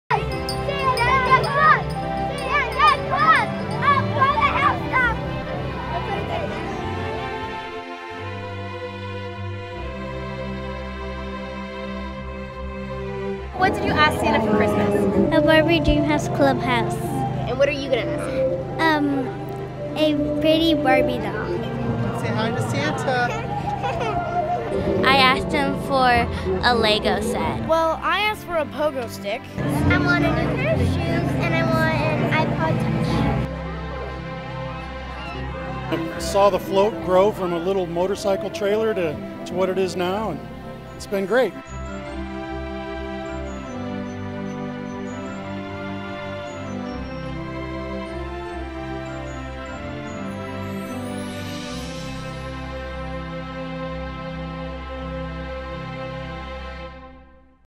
Castaic families welcome Santa in his sleigh
As he’s done for the past three decades, Santa made his way through Castaic on a sleigh-like float Monday night, making frequent curbside stops in the neighborhood.